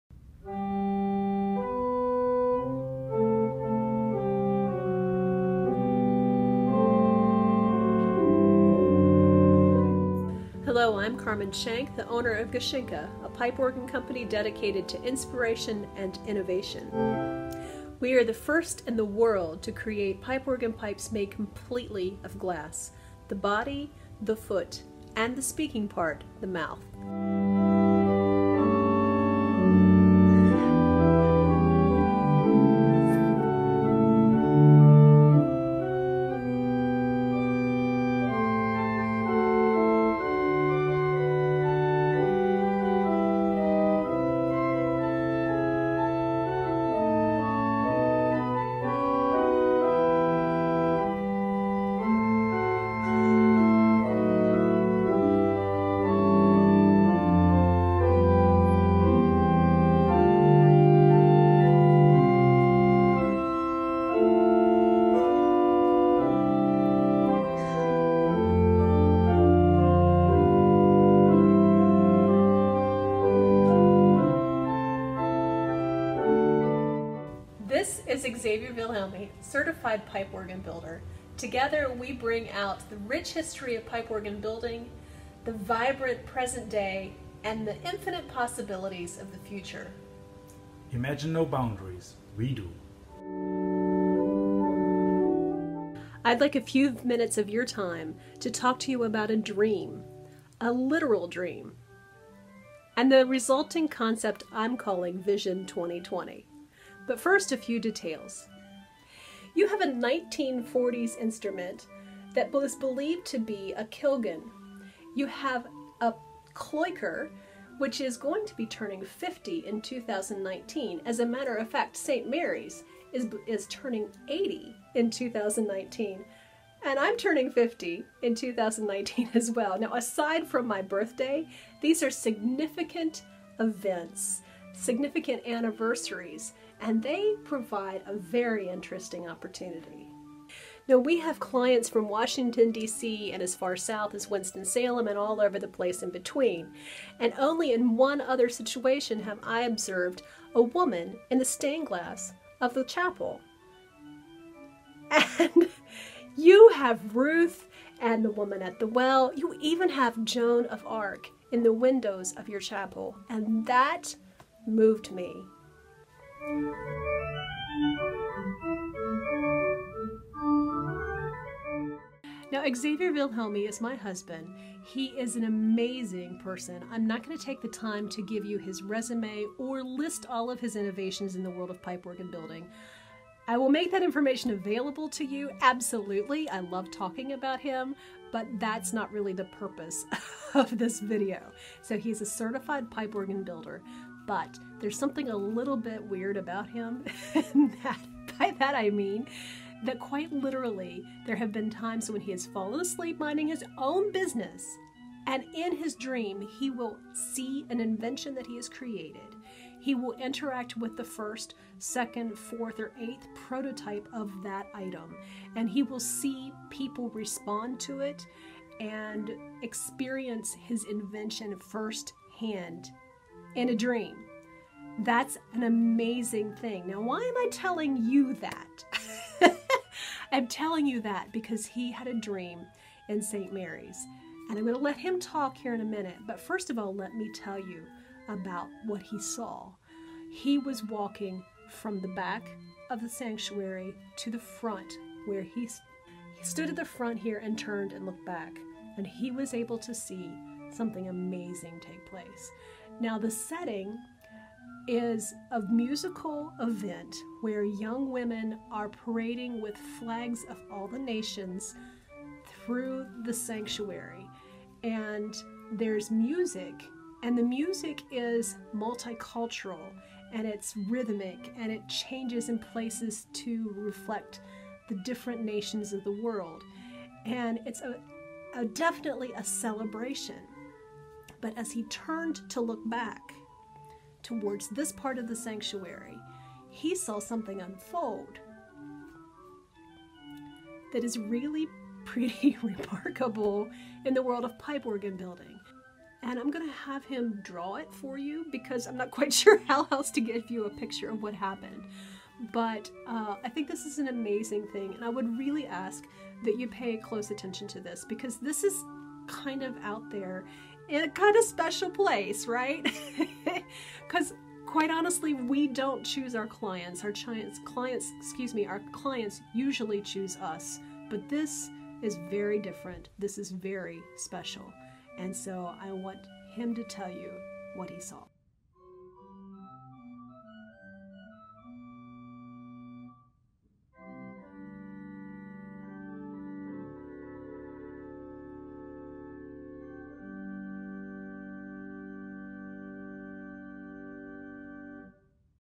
Dreaming Up Innovation: Xaver Wilhelmy's Vision For St. Mary's Chapel At Chatham Hall
I've just discovered these three videos that I had "unlisted" for a number of years.  Thought it might be time to make them public for anyone interested.  Xaver had a invention dream where he walked through St Mary's at Chatham hall.  Listen for yourself, it's pretty amazing!

XaverWilhelmy.com